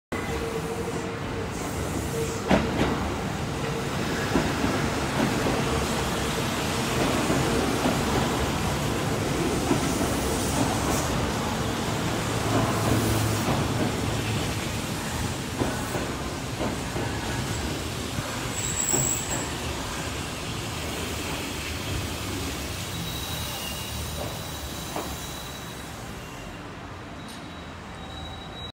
近鉄12400系NN01編成＋近鉄22000系AS08編成 奈良行き 大和西大寺到着
種別『京奈特急』
行き先 奈良ゆき
列車番号 第1807列車
撮影日 5月9日
撮影場所 大和西大寺